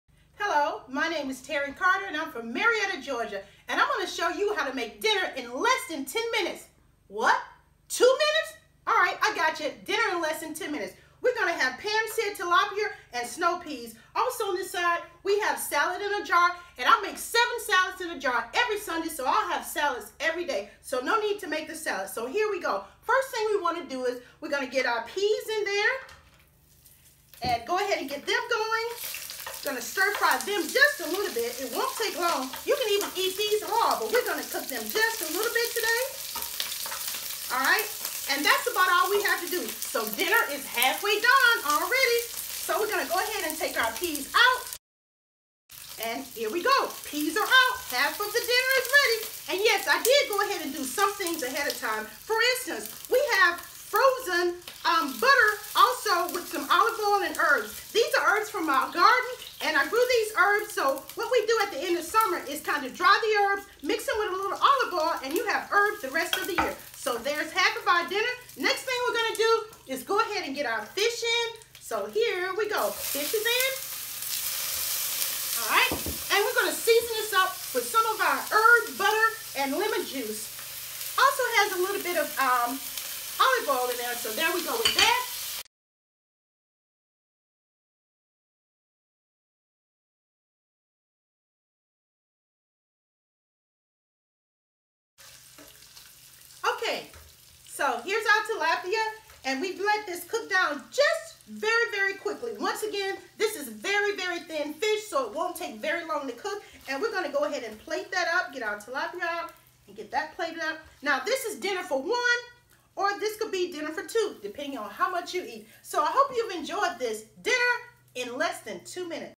Farmer Terri Carter Create TV 2018 Contestant
Create TV’s 2018 Cooking Contestant, Terri Carter is showing you how to make the quickest dinner ever.